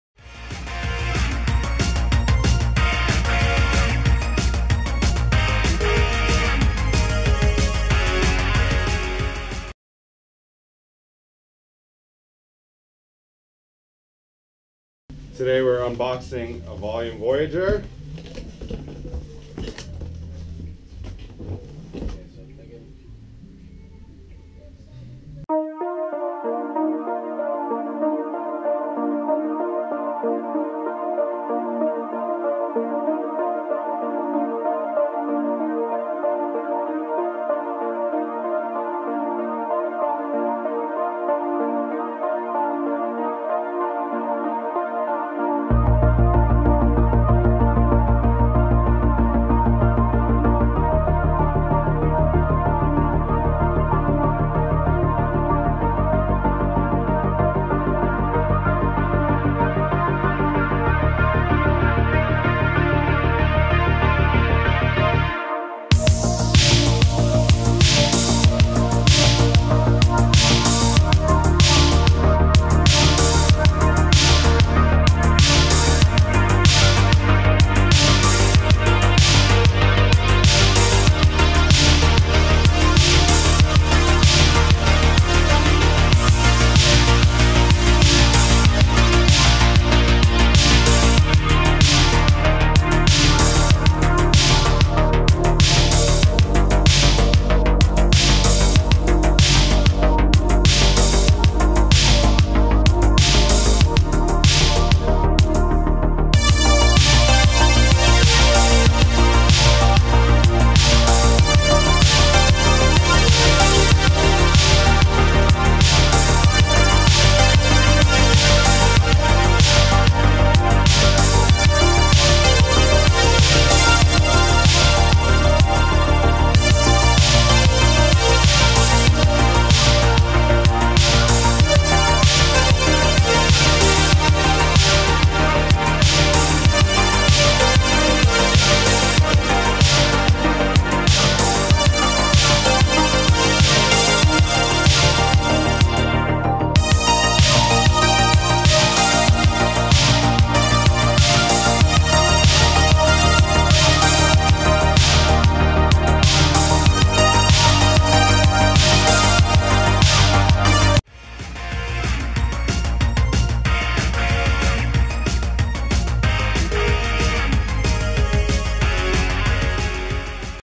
Volume Voyager "Billy Perry" Custom Frame Build @ Harvester Bikes
Voyager Team Frame
Top Tube: 21″
Chainstay: 12.75″-13.25″ (13″ center to center)
BB height: 11.7″
Seat Tube height: 9.25″
Head Tube Angle: 75.25°
Seat Tube Angle: 71°
Seat Post: 25.4
Headtube: Integrated (45°X45° Campy Spec)
Brake Mounts: Removable Seatstay Mounts
Dropouts: Heat Treated, 4mm thick. Clears hub guards and pegs of all sizes.
Weight: 5 lbs 0 oz (21″)